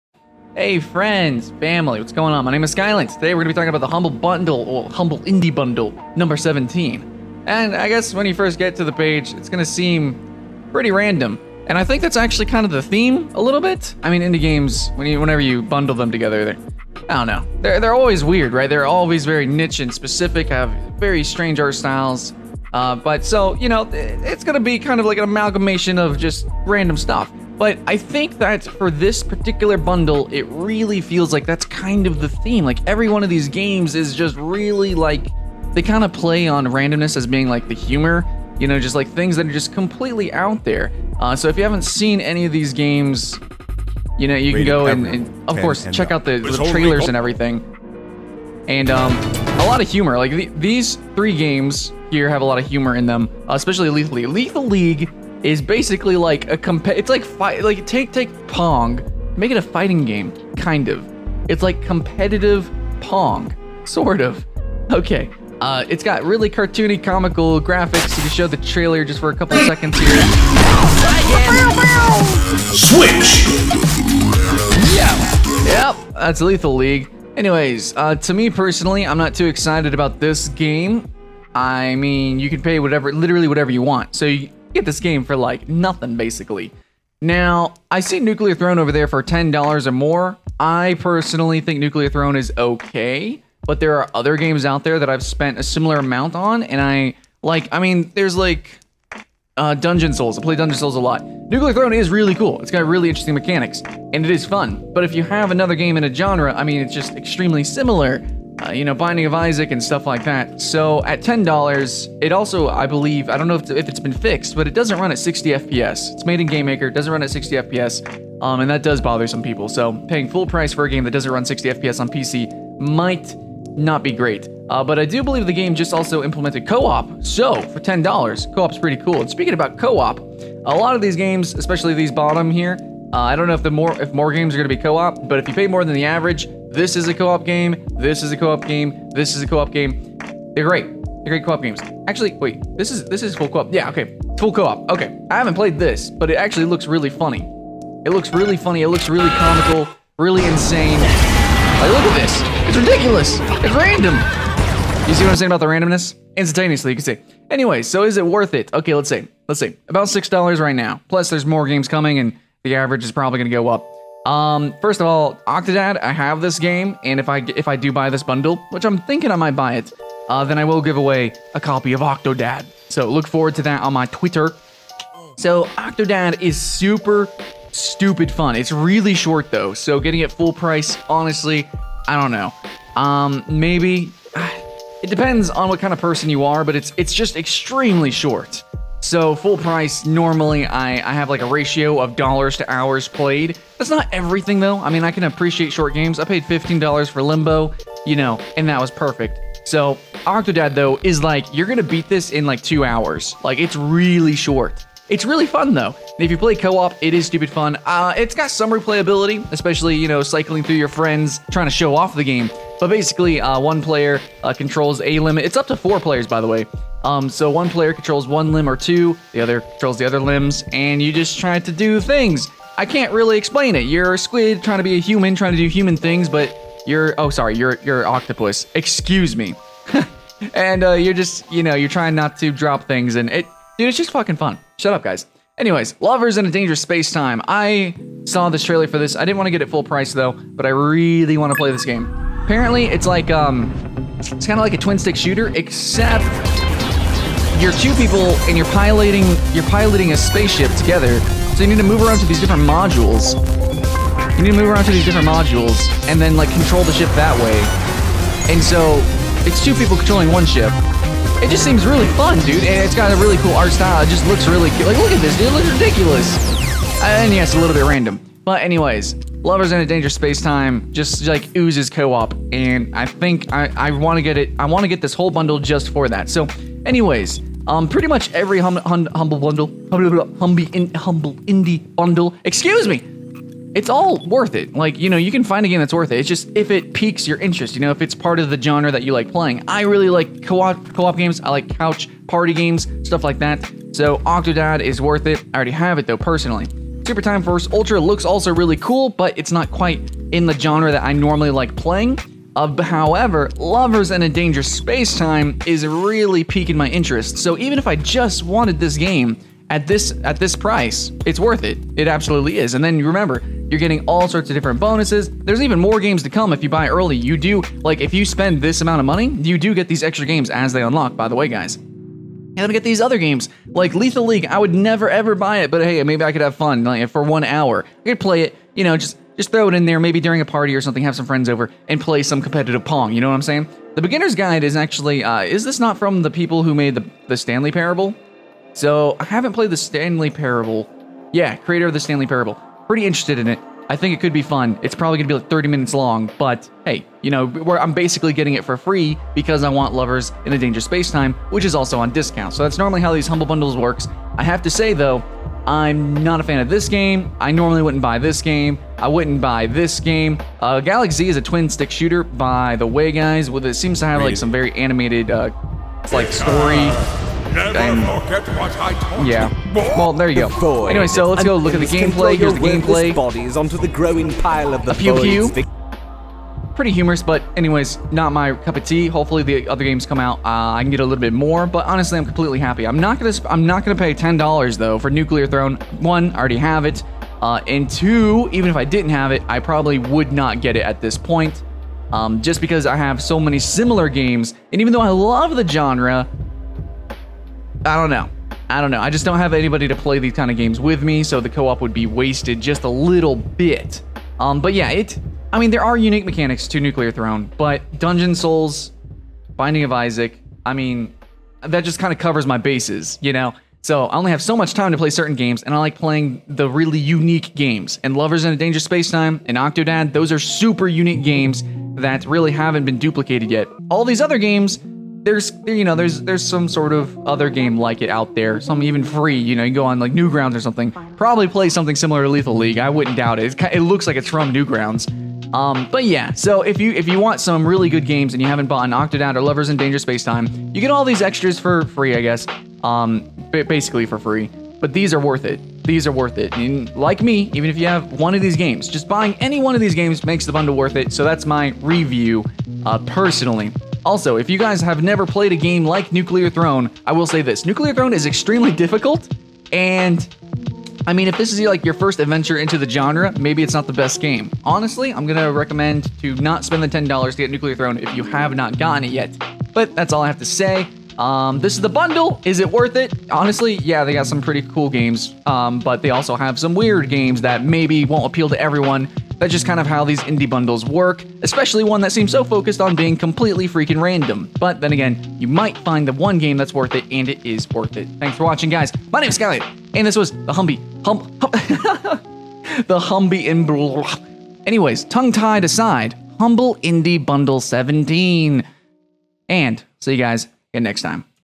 Humble Indie Bundle 17 Review Live Browse and Buy
I got the bundle friends, go to my twitter @skylent to get code for Octodad!